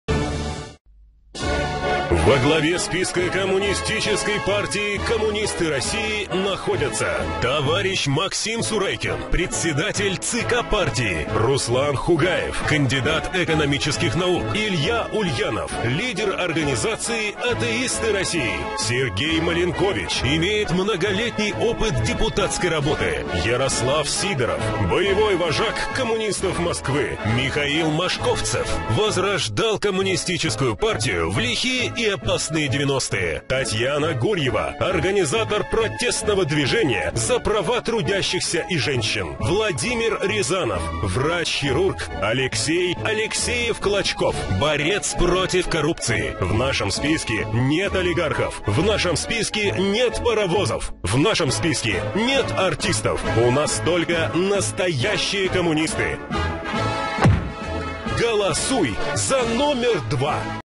политическая реклама Коммунисты России. КПКР. 2016 г.
Выборы в Государственную Думу. Лидеры списка КПКР.